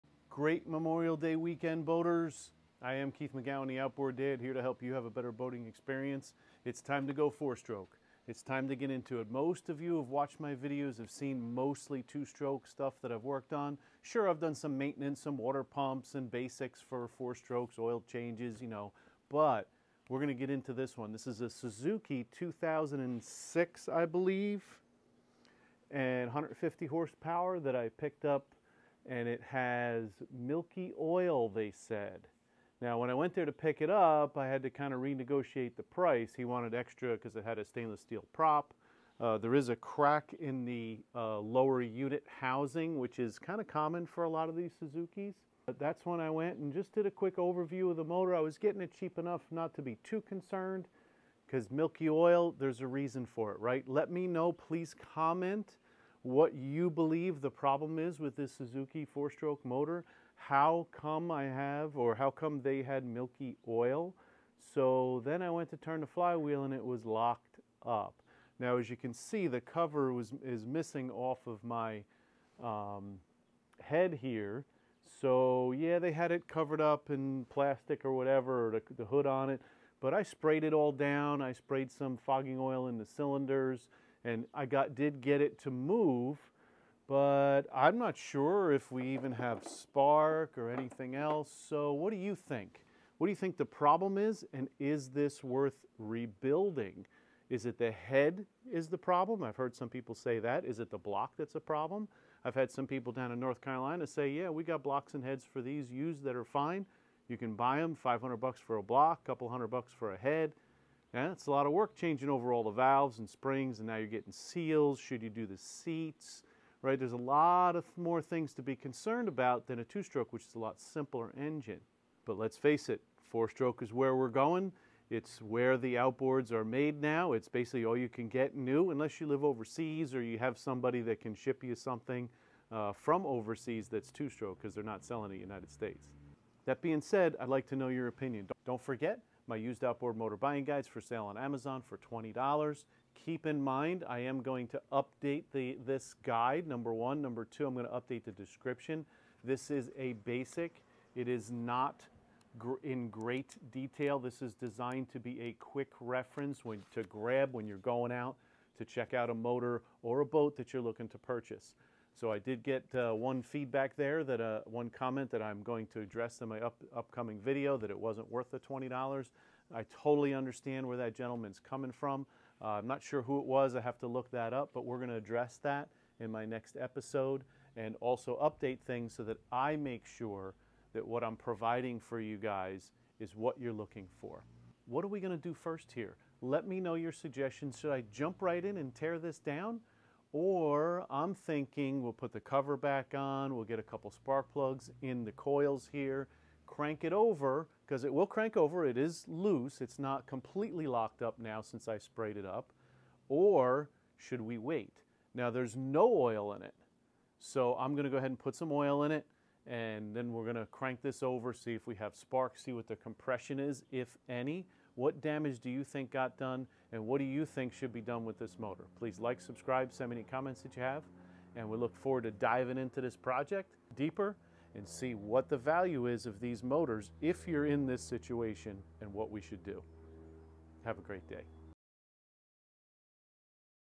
“Rebuilding a 4-Stroke vs. 2-Stroke Outboard- Is It Even Worth It?”
Kicking off the 2-stroke vs 4-Stroke series with a big question: Is it worth rebuilding this four stroke Suzuki 150? Let me know what you think before I start testing. The difference between these motors are bigger that most people realize